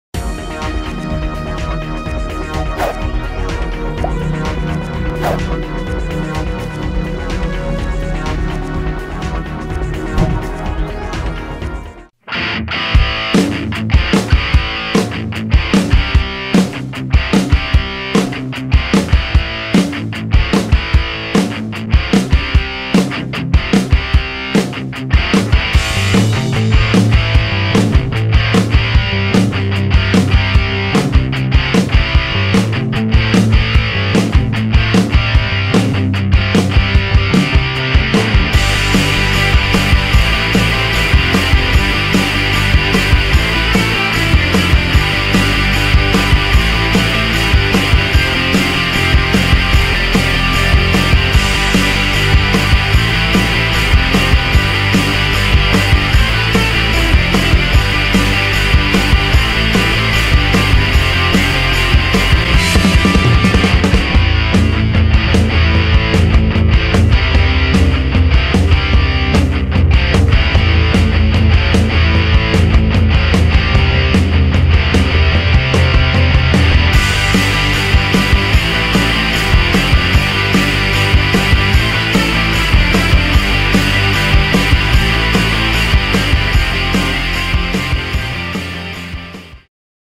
Demo Reel 2015
Shot mostly on my FS100 with a few clips from my A6000. Edited in Sony Vegas Pro 12.
I used a bunch of different grades when doing colour in order to show that I can shoot in and grade for a variety of looks and styles.